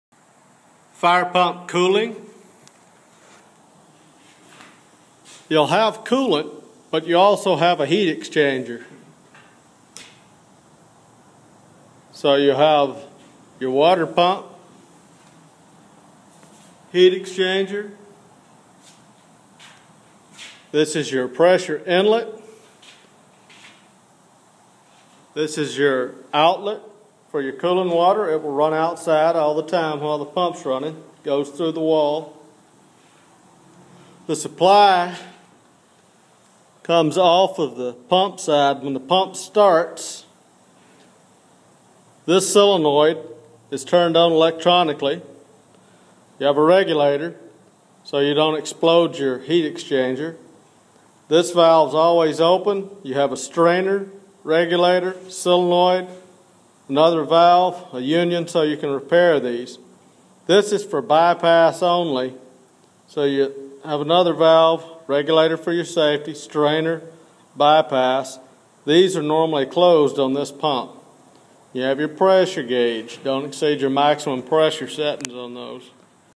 Diesel pump cooling system
Heat exchanger, solenoid a, and strainers